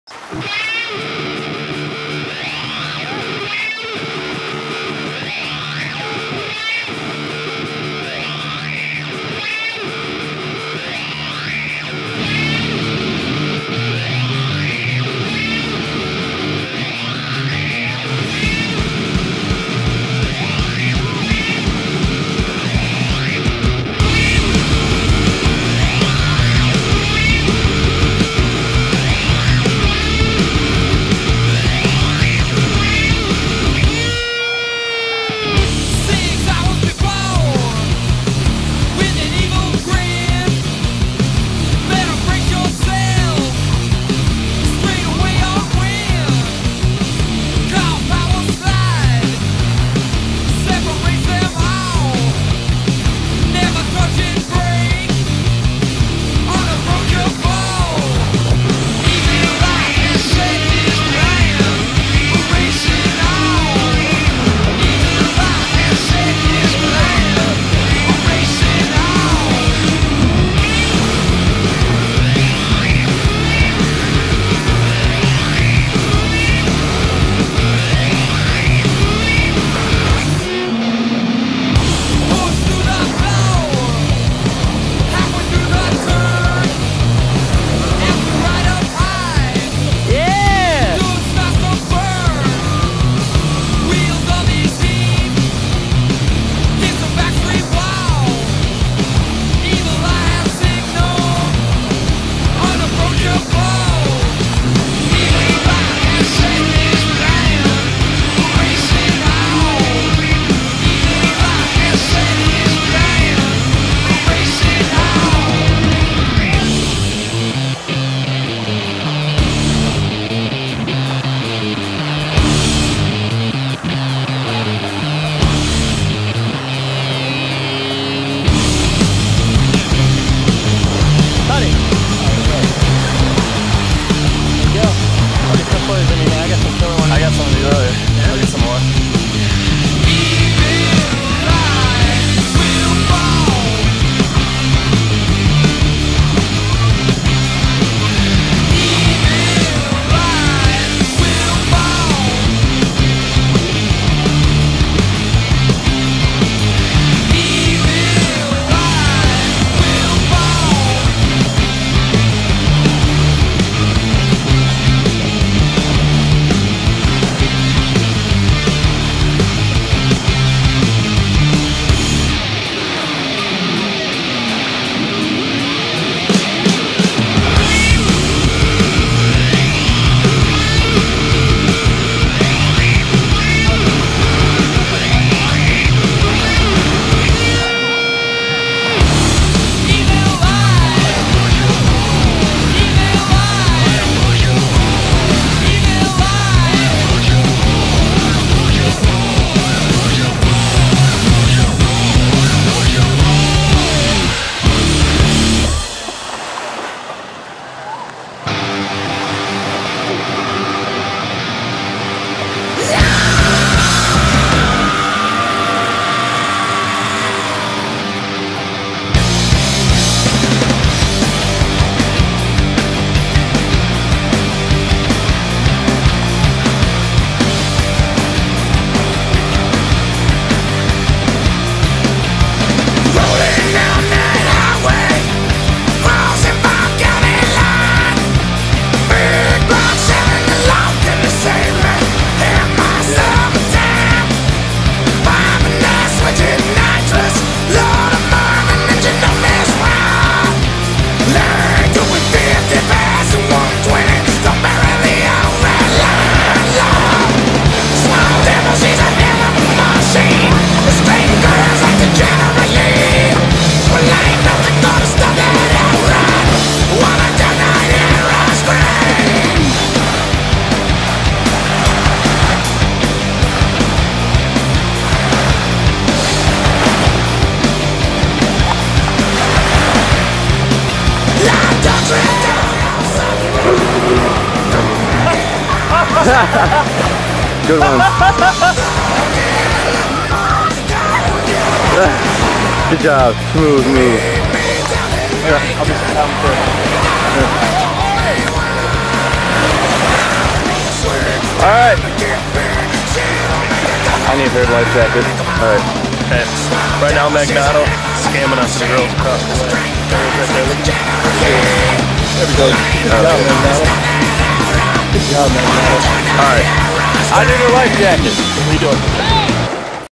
Memorial Day Weekend Jet Ski Party Time
Me, Aaron, and Hoffman partying @ Edgewater beach cleveland america.